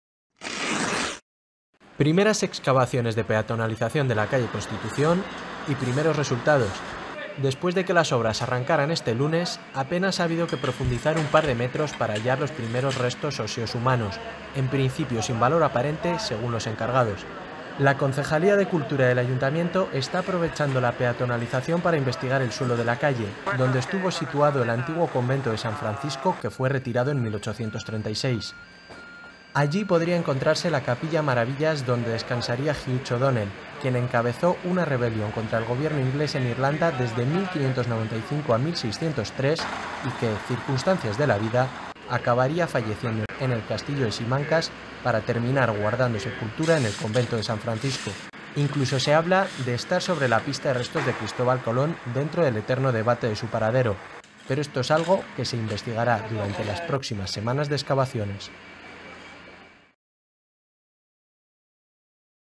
Las primeras excavaciones en la calle Constitución revelan huesos humanos “sin valor aparente”
Todo se remonta al siglo XIII, momento en el que el Convento de San Francisco fue fundado en Valladolid. Sobra decir que por aquel entonces la ciudad era muy diferente a como es ahora. Este espacio se encontraba ubicado en la actual calle de la Constitución, con una cercana Plaza del Mercado que a la postre terminaría siendo la actual Plaza Mayor. La cuestión es que el Ayuntamiento, más concretamente la Concejalía de Cultura, ha decidido aprovechar la tesitura de la peatonalización de la calle Constitución estos días para indagar sobre esta antigua Capilla.

Las obras, que han arrancado oficialmente este lunes, durarán cerca de tres meses  y los quince primeros días supondrán la búsqueda de la capilla funeraria ‘Maravillas’, parte del mencionado Convento de San Francisco. Y poco tiempo se ha tardado en dar con resultados, apenas un par de metros de búsqueda hacia abajo.
Este martes a primera hora aparecían unos restos óseos humanos “sin valor aparente”, según ha confirmado a Tribuna de Valladolid el encargado del proyecto a cargo de la Concejalía, Carlos Burgos. “Hay que tratarlos debidamente porque no dejan de ser restos de personas, pero no es lo que andamos buscando inicialmente”, explica.

“Hay que analizarlos, parece que se trata de rellenos de muchos años de movimiento ahí debajo. Pueden ser del siglo pasado, o de hace dos siglos… La desamortización que tiró abajo el Monasterio es de 1836, o sea que podríamos estar hablando de restos de estos años”, sigue. Y es que el espacio religioso ocupaba prácticamente la totalidad de la actual calle Constitución, con hasta cinco capillas en su haber.

¿Pero entonces qué se busca exactamente? Pues a Red Hugh O'Donnell, quien encabezó la rebelión contra el gobierno inglés en Irlanda que acabaría provocando la Guerra de los Nueve Años, una revuelta contra los ingleses que duró desde 1595 a 1603… y que, circunstancias de la vida, acabó falleciendo en el Castillo de Simancas para terminar guardando sepultura en el Convento de San Francisco.

Incluso se cuenta con poder encontrar pistas de Cristóbal Colón, dentro del eterno debate sobre el paradero de sus restos después de su muerte en Valladolid. Pero para eso habrá que seguir investigando en los próximos días e incluso semanas.